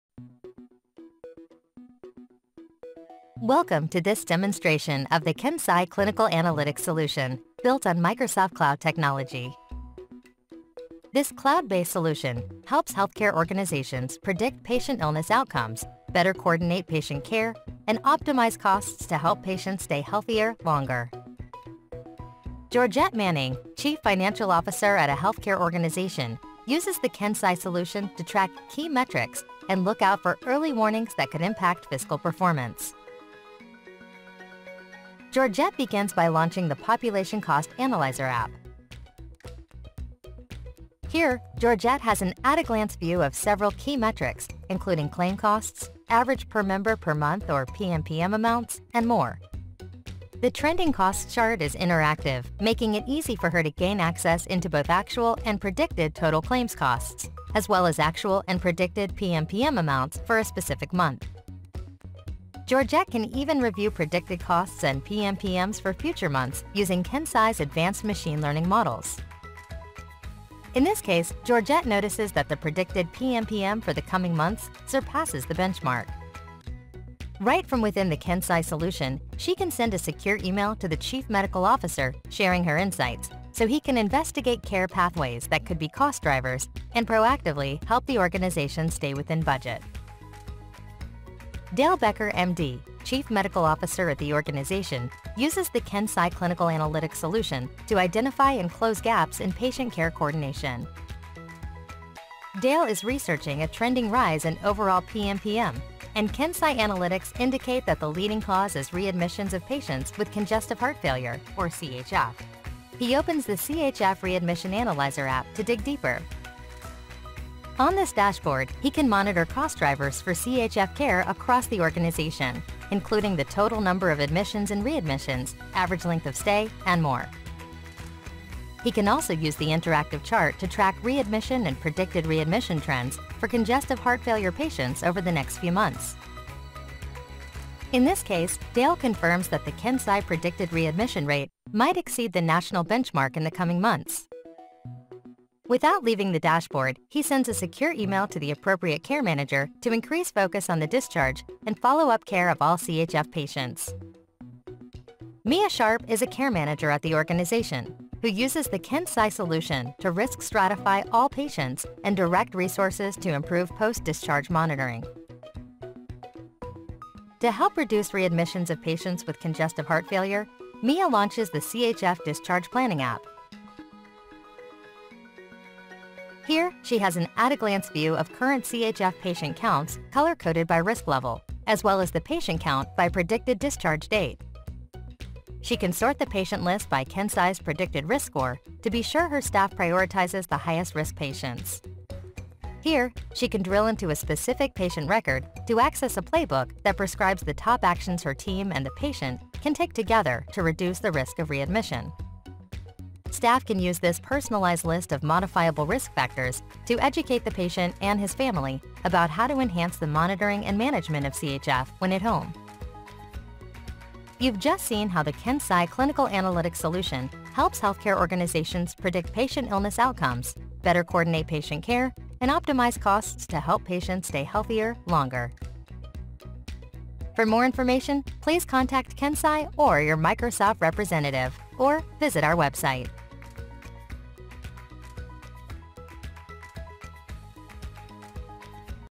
KenSci Clinical Analytics on Microsoft AppSource
KenSci Clinical Analytics — What and Why?

Sepsis accounts for considerably more hospital readmissions and associated costs than any of the four medical conditions tracked by the federal government to measure quality of care and guide pay-for-performance reimbursements, per an analysis led by the University of Pittsburgh School of Medicine and VA Pittsburgh Healthcare System.

KenSci's Clinical Analytics solution helps CFOs and CMOs reduce the total cost impact of Sepsis and HAC by helping to identify patterns and predict patients who are at high risk. Enabling care managers to engage and modify their risk helping to prevent infection, readmission and mortality.